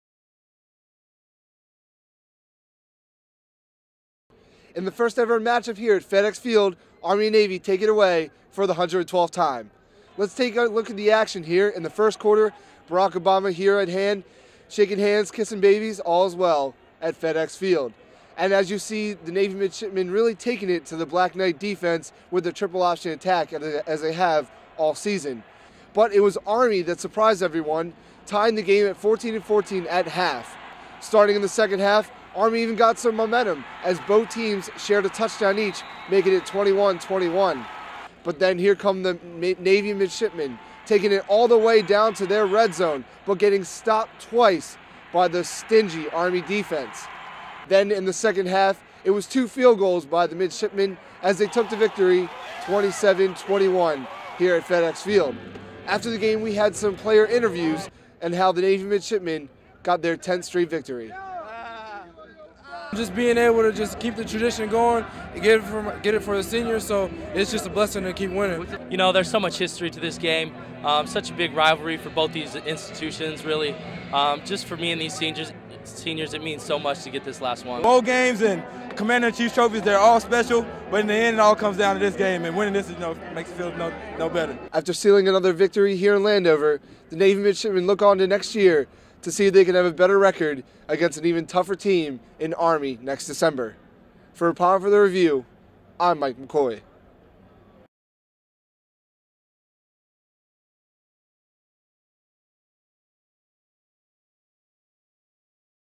Army Navy Game Coverage Mike McCoy
Mike McCoy covers the Army/Navy Game live after the game with player interviews